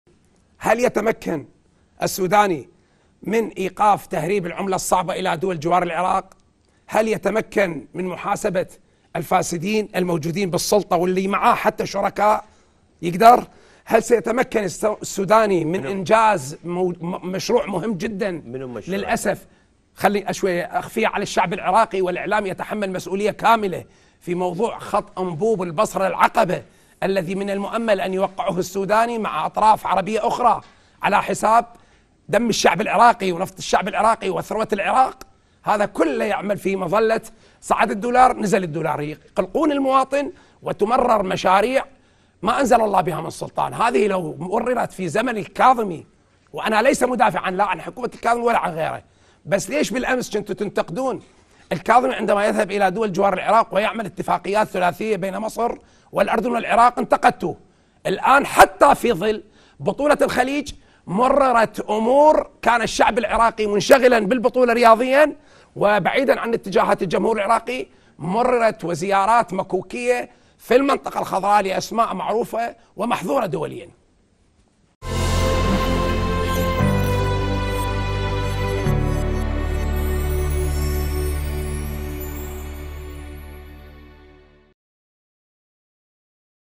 في تصريح ناري .. فتاح الشيخ: تم بيع العراق خلال الفترة الماضية واشغال الشارع بخليجي 25 وأزمة الدولار
في تصريح ناري .. فتاح الشيخ لـ"نفس عميق": تم بيع العراق خلال الفترة الماضية واشغال الشارع بخليجي 25 وأزمة الدولار

11258📺 افقي